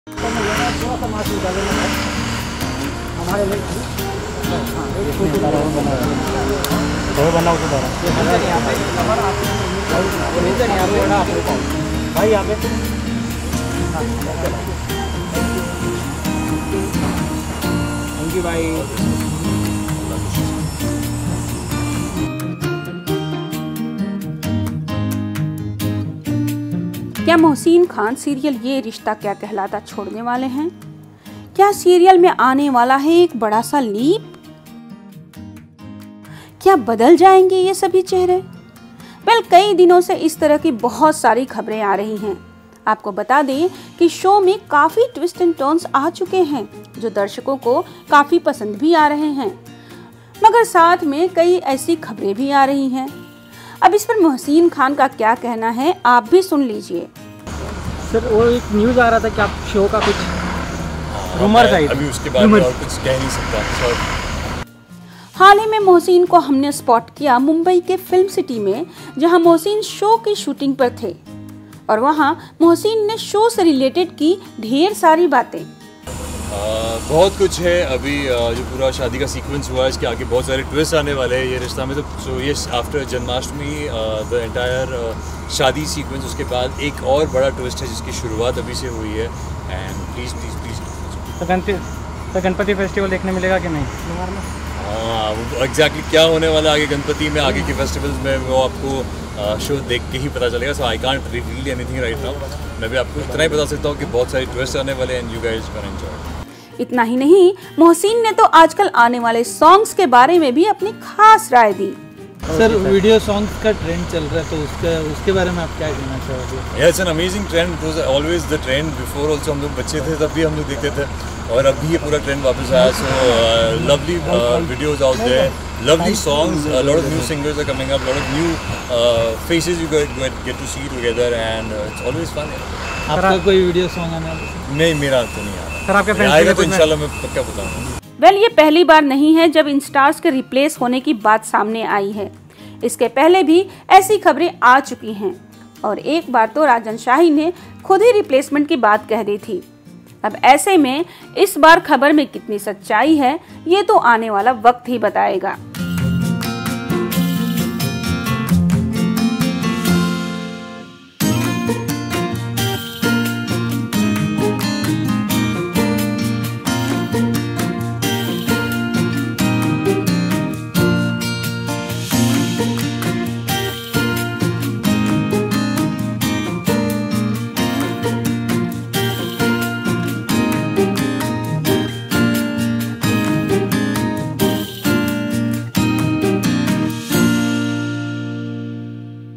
Is Mohsin Khan aka Kartik leaving popular TV show Yeh Rishta Kya Kehlata Hai?
Mohsin Khan is in rumours to be leaving his show Yeh Rishta Kya Kehlata Hai as there are reports that the makers are planning to take a leap in the show and there will be a change of cast. There have been already many twists and turns in the show to keep the fans entertained through the years. Mohsin Khan aka Kartik talks about his popular TV show Yeh Rishta Kya Kehlata Hai. He also refused to talk about the rumour when he was spotted by the paparazzi. He also talks about Ganesh Chaturthi celebration in the show. He gives his opinion about music videos and the ongoing trend. Watch the video to know more.